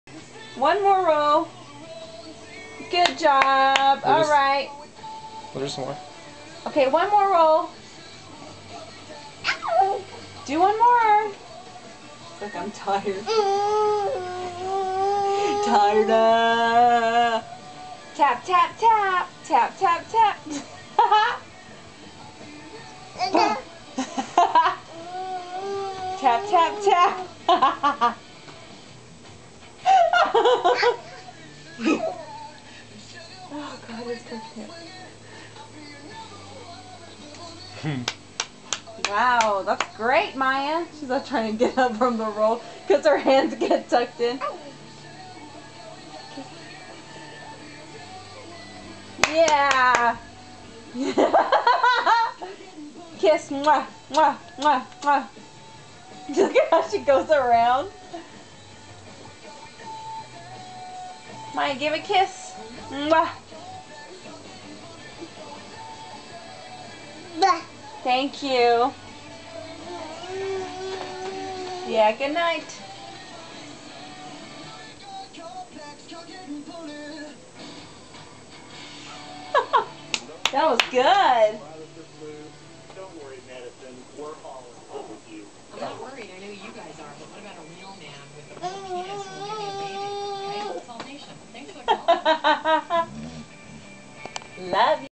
Rolling_Girl.avi
A clip of my 16 month old daughter Maya doing some rolls on the bed.